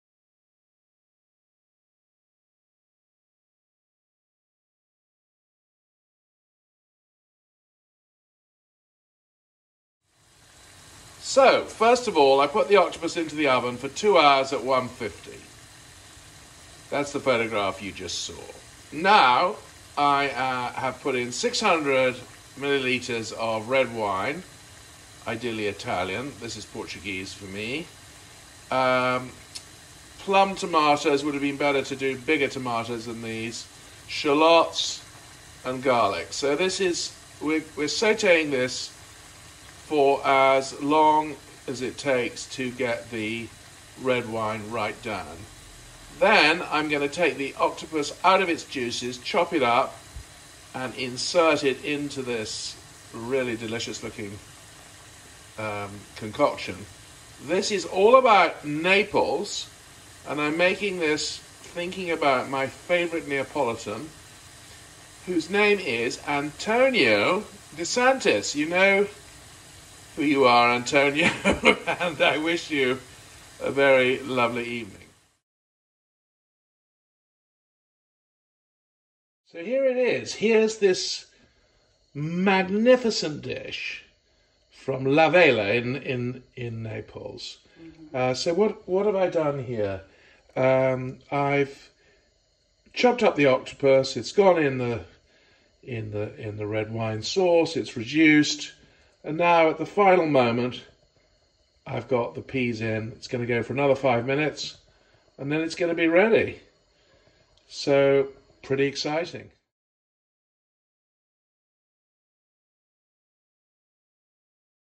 Octopus in Red Wine!
Octopus in red wine - Naples Style!

History

La Vela is all about Octopus!  Italy is all about red wine!

Ingredients

One 750g octopus
600ml red italian wine
Pinch of demerara
Bag of frozen peas
2 plump sliced garlic cloves
6 sliced shallots
4 plum toms
Chopped flat leaf
Copious olive oil

Method

Chuck octopus into oven smothered in olive oil for 2 hours at 150

Sizzle garlic in 50 ml olive oil add toms and shallots. Sauté gently. Add red wine reduce by 90%

Add chopped up octopus, and 100ml water. sauté for 30 mins

Add peas for 5 mins